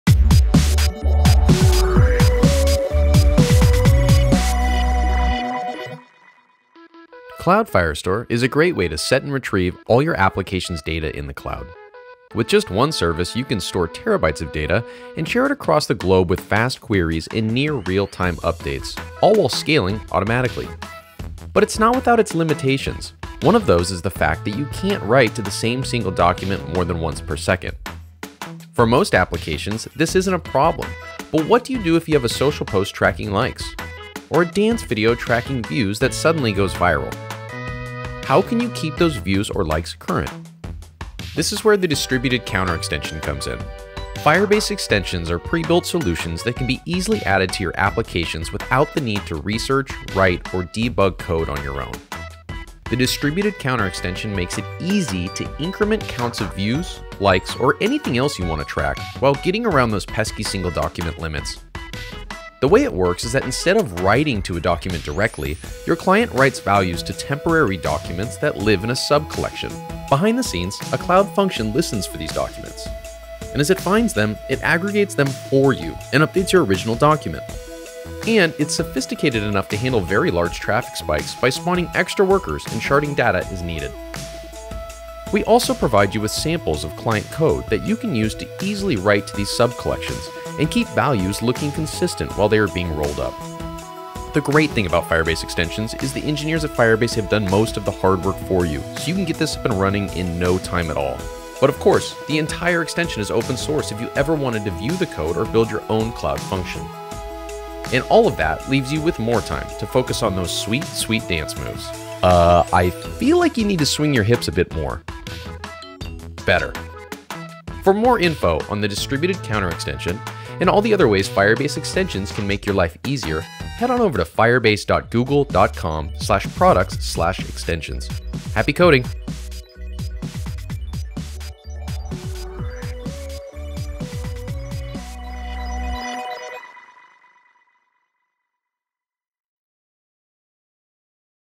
Meet an Extension: Distributed counter
Cloud Firestore is a great way to set and retrieve all your application's data in the cloud. There is a limit of one write per second per document, so how can you keep track of things like views or likes on a rapidly growing viral video? This is where the distributed counter extension comes in.

product: Firebase - Extensions; re_ty: Publish;